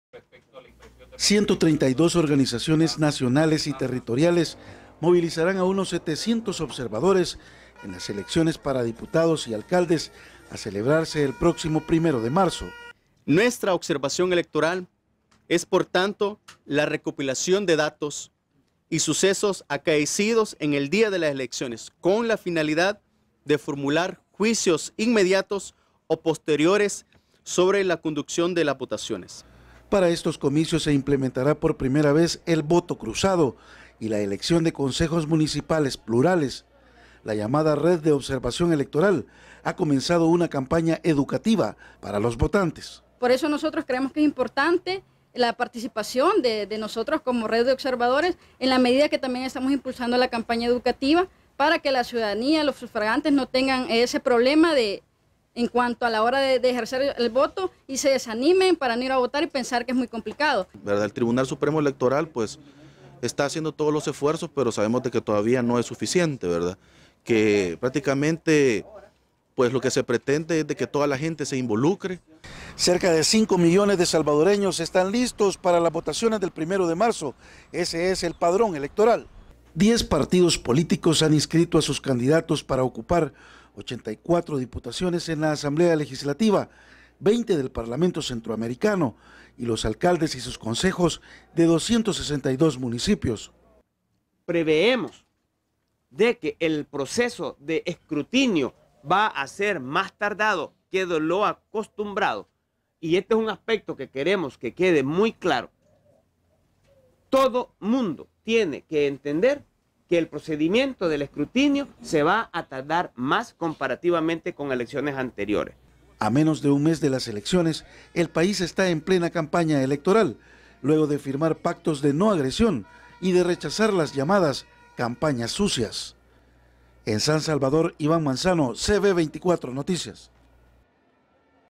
El Salvador aprobará 700 observadores para elecciones legislativas y municipales
En El Salvador,  con el objetivo de lograr unos comicios transparentes, participativos y evitar fraude, la Red de Observación Electoral anunció que esta lista para aprobar 700 observadores para las elecciones legislativas y municipales del 1ro de marzo.